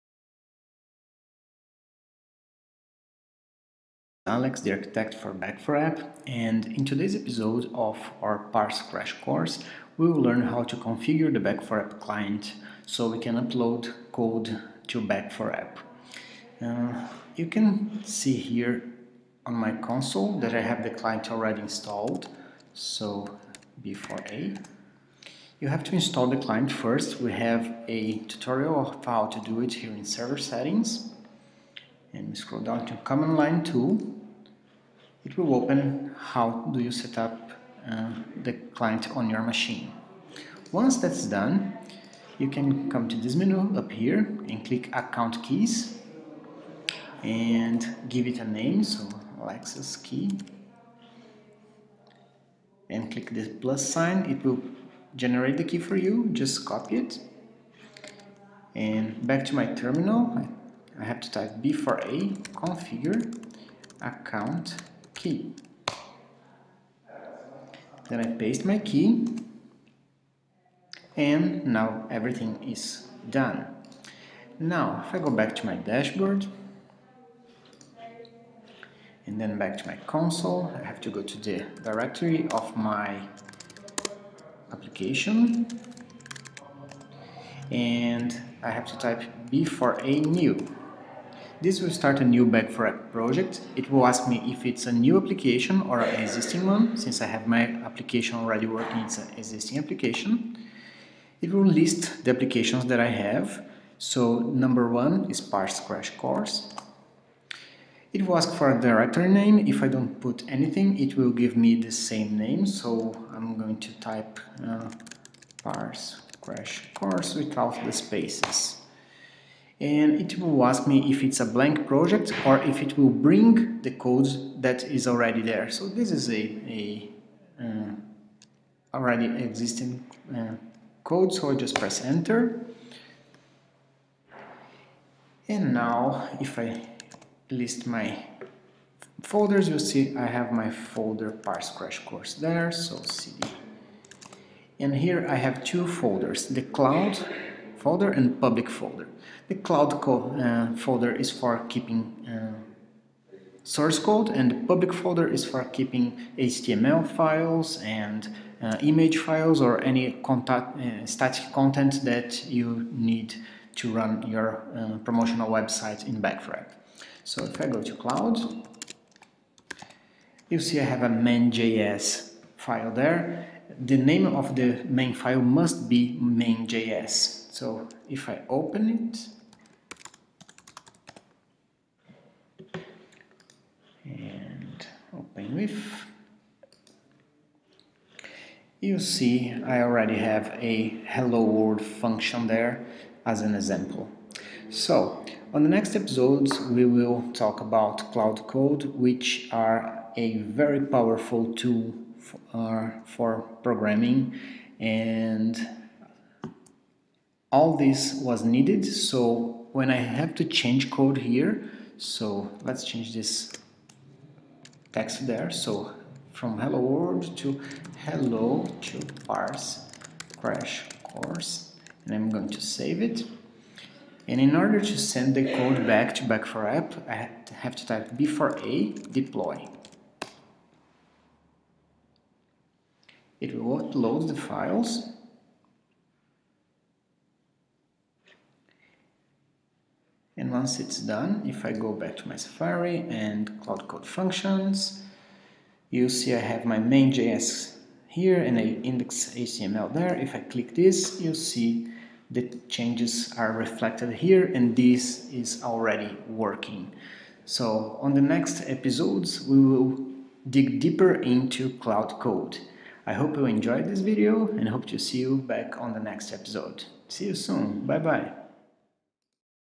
Parse Crash Course #22 Configuring the Command Line Interface on Parse
In this video, you'll learn how to configure the Back4app CLI

Release your app faster and better, sign up to Back4App, the complete open source serverless backend platform.